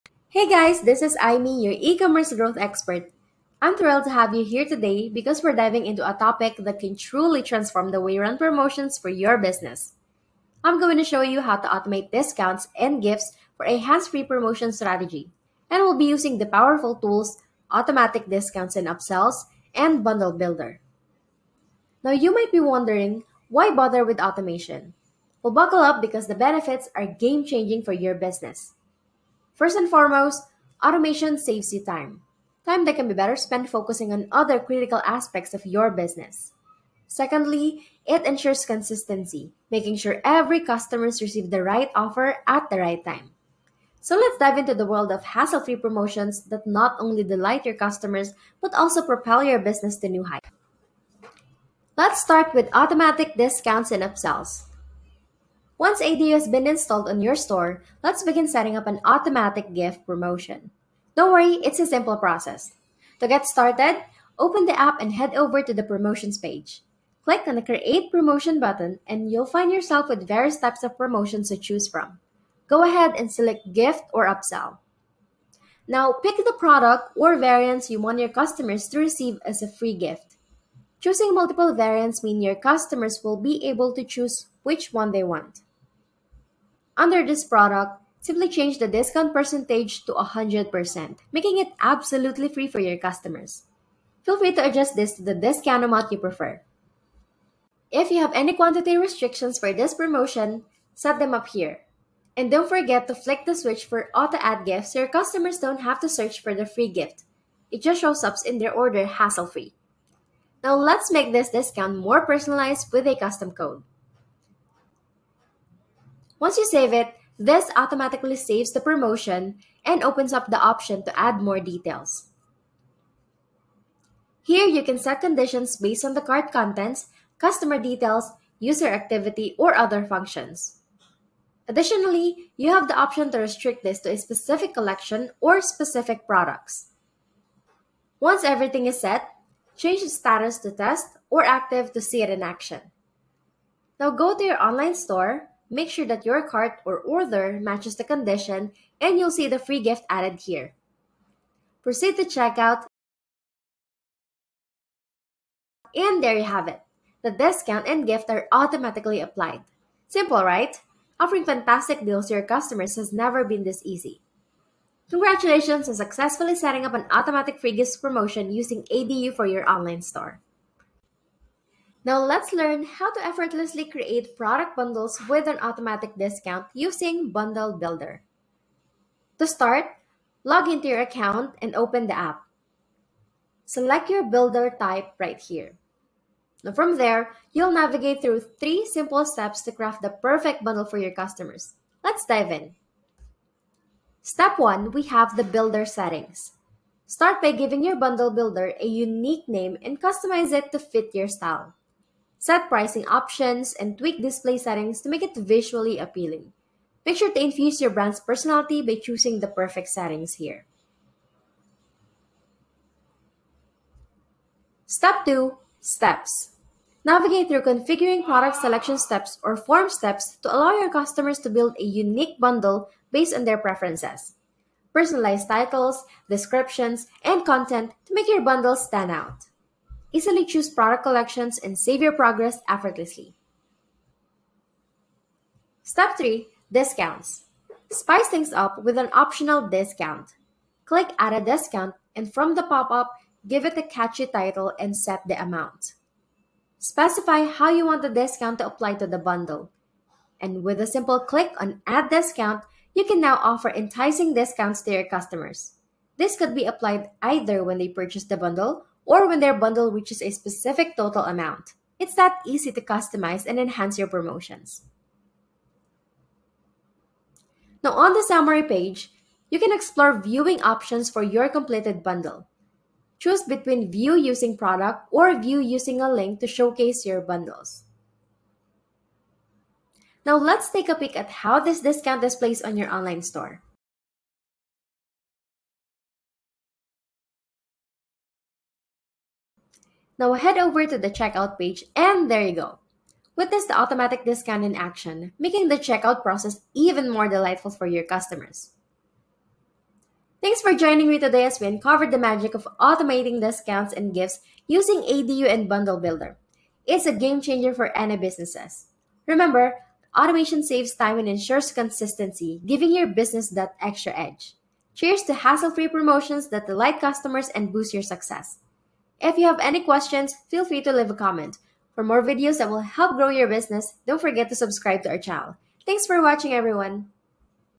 Mastering Automated Discounts and Gifts for Your Ecommerce Store
Transform your e-commerce game with our latest video! Explore the magic of automated discounts and gifts using Shopify apps like Automatic Discounts & Upsells and BundleBuilder. Learn step-by-step strategies to boost sales, enhance customer satisfaction, and streamline your store's offering. Watch now to discover how these powerful tools can elevate your e-commerce experience and drive success for your business. Don't miss out on the future of effortless promotions and bundles – tune in today!